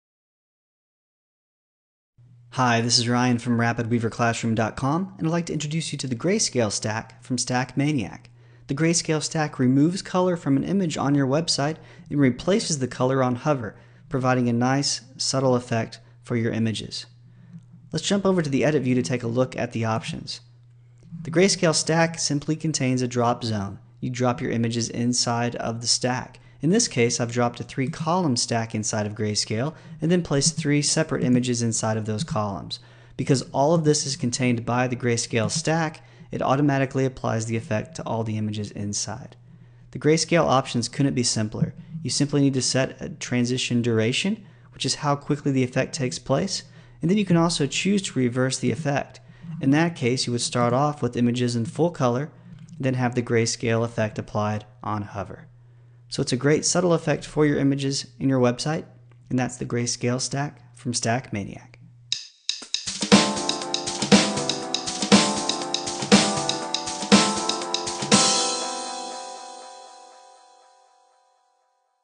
GrayScale from Stack Maniac
Overview of the image hover fx stack, Grayscale, from Stack Maniac - for RapidWeaver.
Overview by Ryan Smith from RapidWeaverClassroom.com